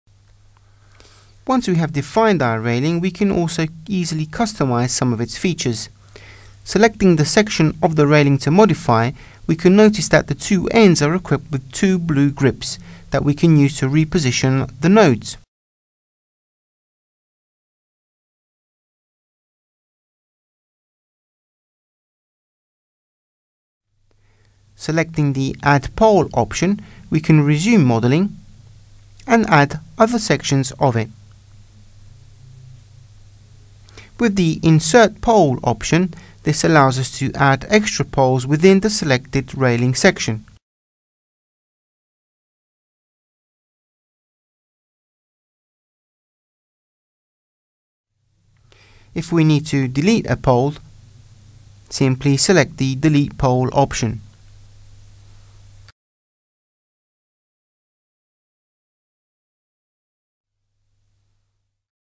Edificius Tutorial - Editing a Railing - ACCA software
Let's learn more about Editing a Railing using Edificius BIM software. Learn more about Modify the geometry of a railing: adding, inserting or deleting struts and poles in a 3D BIM model the easy and quick way. Find out more about Edificius and download the Trial version and discover its powerful BIM design tools .